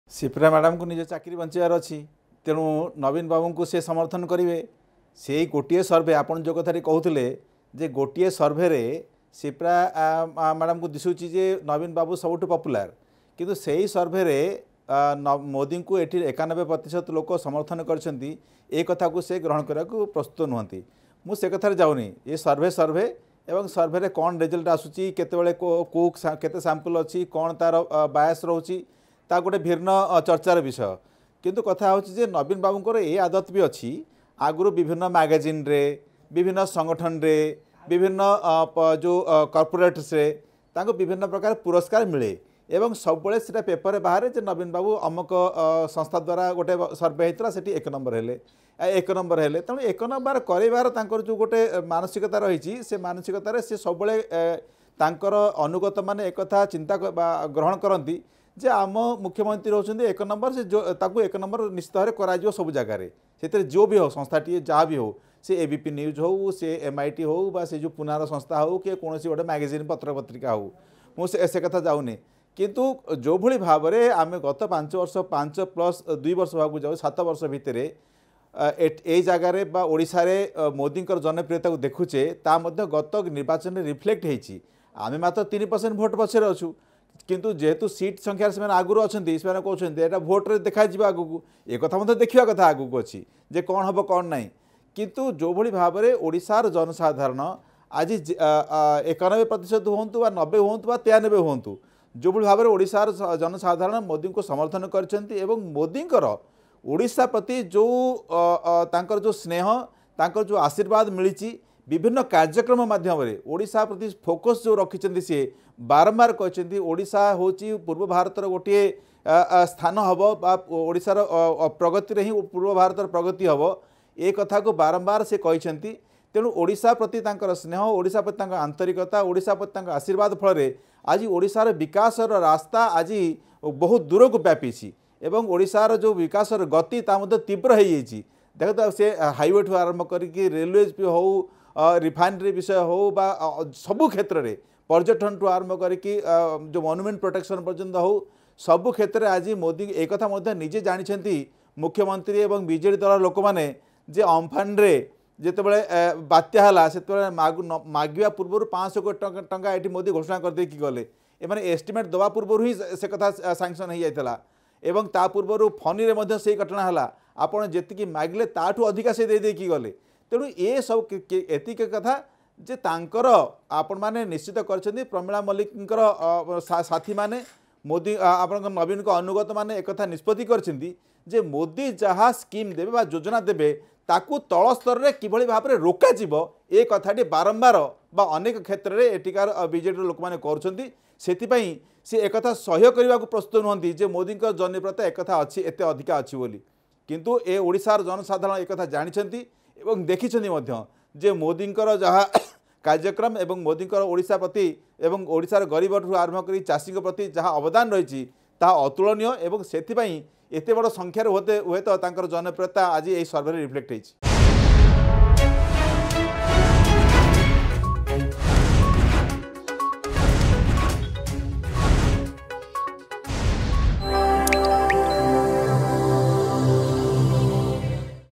ଓଡିଶାର ଜନତା ମୋଦିଙ୍କ ସମର୍ଥନରେ ଅଛନ୍ତି
ଓଡିଶାରେ ମୋଦିଙ୍କ ଯୋଜନାକୁ କେଉଁଭଳି ରୋକାଯିବ ତାର ବ୍ୟବସ୍ଥା ସବୁବେଳେ ବିଜେଡିର ଲୋକମାନେ କରୁଛନ୍ତି ବୋଲି ପ୍ରତିକ୍ରିୟା ରଖିଲେ ବିଜେପି ମୁଖପାତ୍ର ଜତିନ୍ ମହାନ୍ତି.
ଅଧିକ ଖବର ପାଇଁ ଡ଼ାଉନଲୋଡ କରନ୍ତୁ “Argus News” ଆପ୍